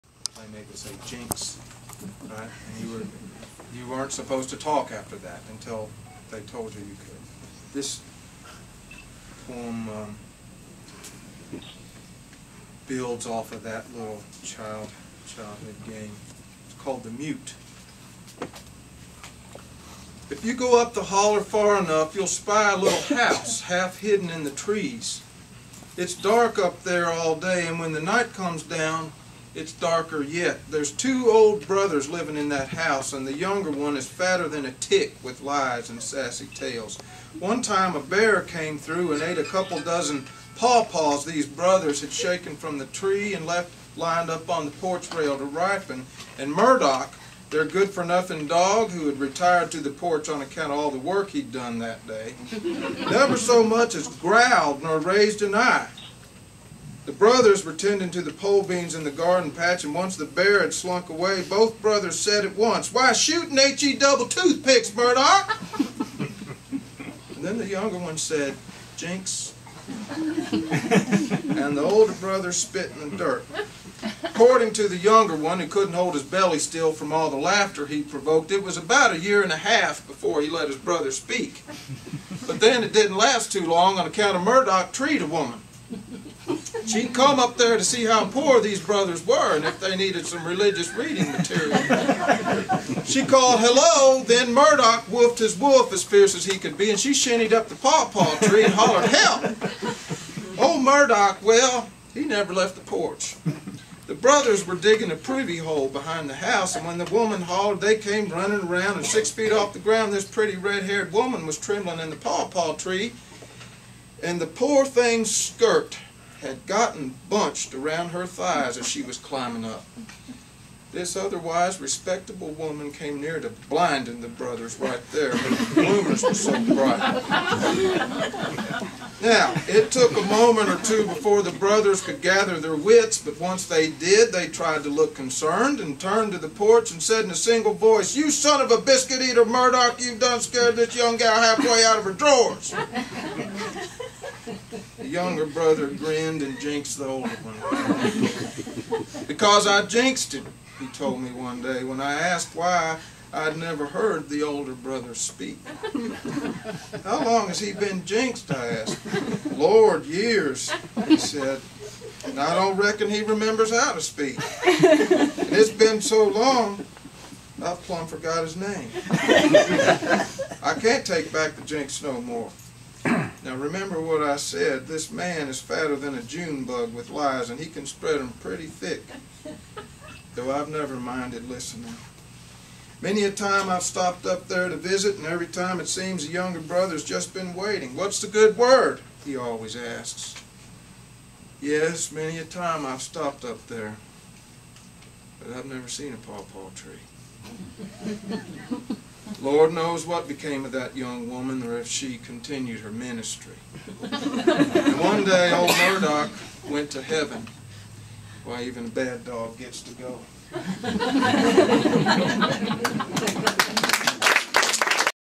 Mauice Manning - The Mute - April 2010.avi
Poet Maurice Manning reads his poem THE MUTE, from his collection THE COMMON MAN.  This reading occurred at The Morris Book Shop in Lexington, Kentucky, April 2010.